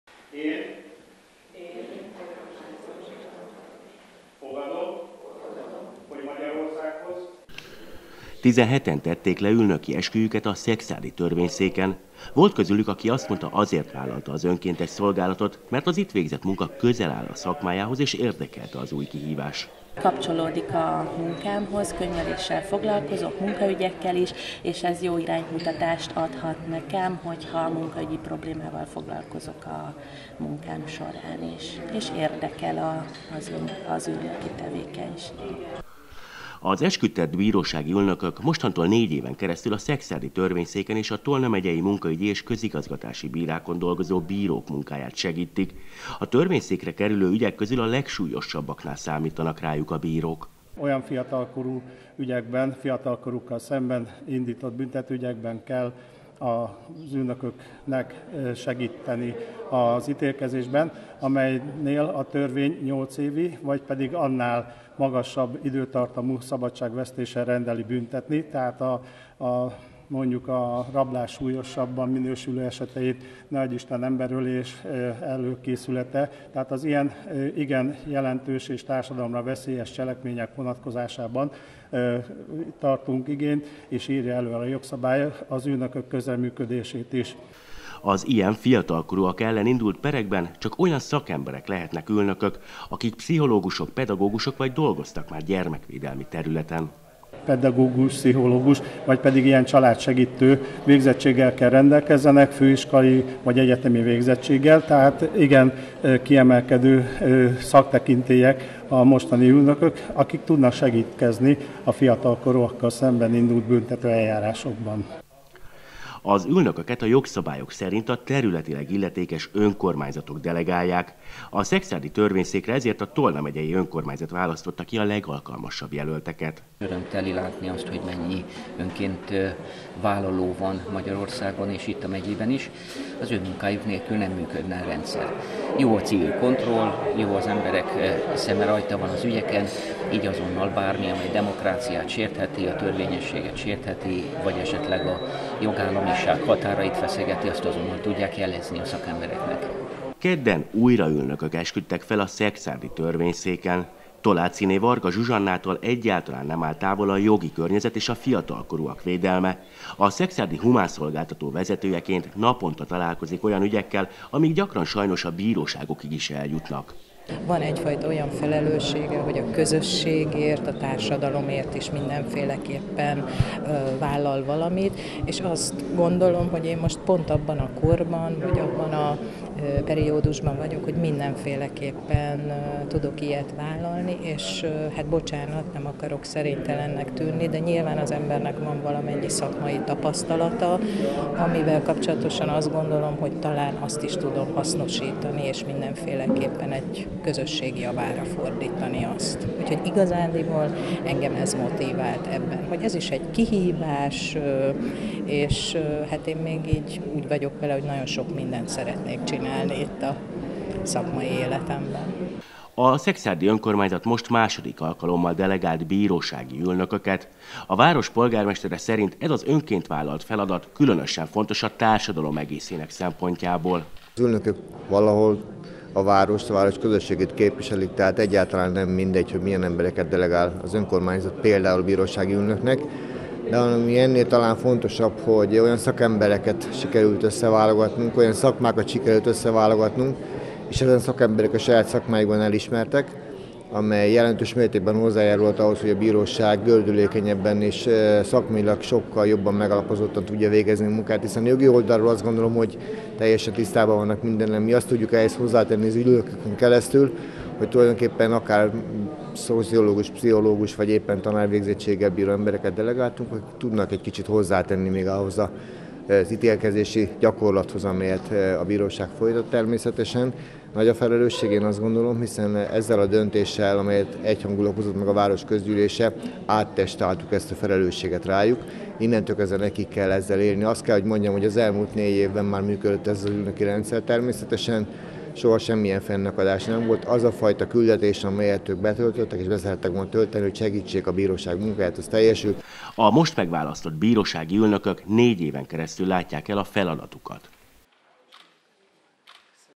Bírósági ülnököket választottak a Szekszárdi Törvényszékre 20190603
Bírósági ülnököket választottak a Szekszárdi Törvényszékre, a Szekszárdi Járásbíróságra és a Tolna Megyei Munkaügyi és Közigazgatási Bíróságra. Ők négy éven keresztül segítik majd a bírók munkáját. A kiválasztásuk nagyon szigorú feltételek mentén történt, nem véletlenül ugyanis mások mellett a fiatalkorú vádlottak ellen indult legsúlyosabb büntetőügyekben lesz szerepük.